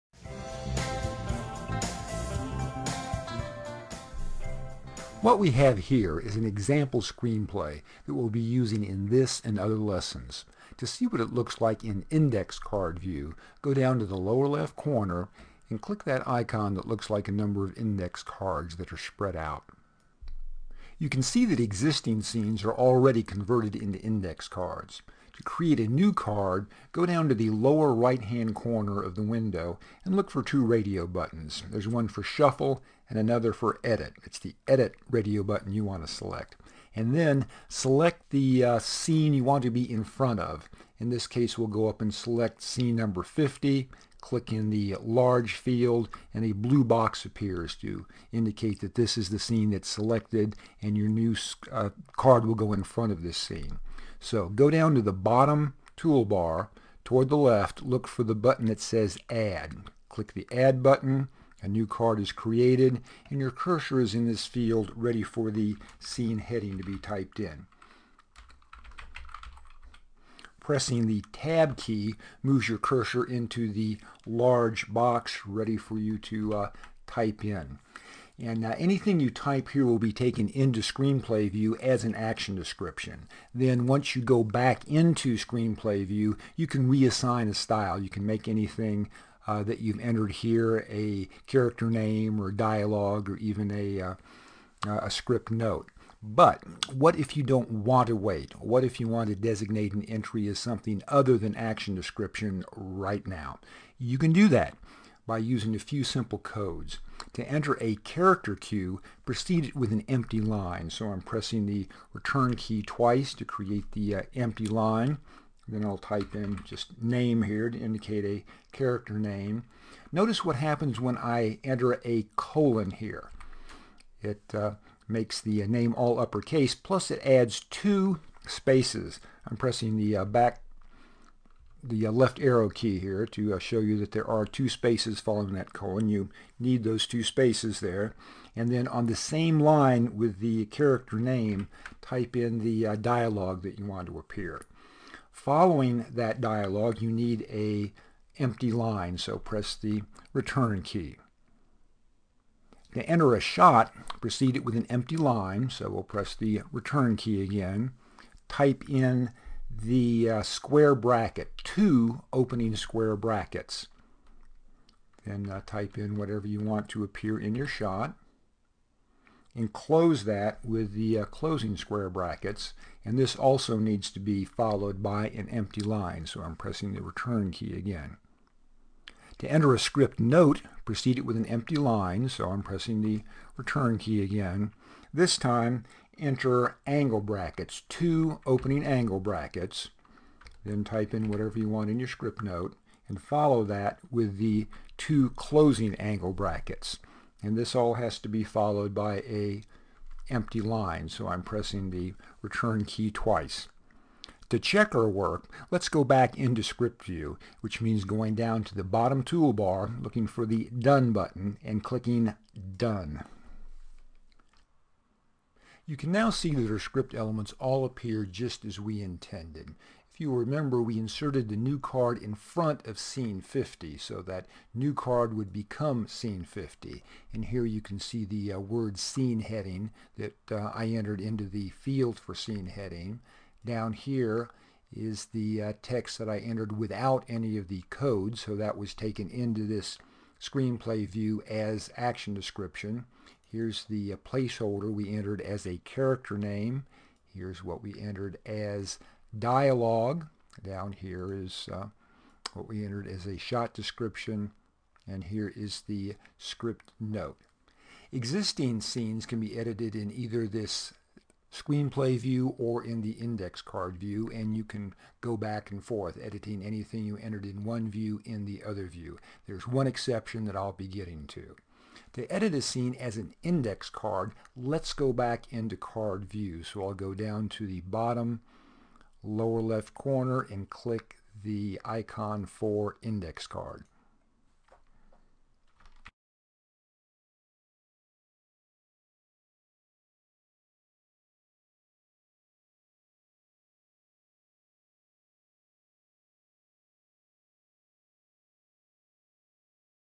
Using Index Cards: Open Index Cards 1/3
In this lesson, you will learn how to open the Index Card View in Movie Magic Screenwriter.  You will also learn how to outline and write your screenplay using Index Card View.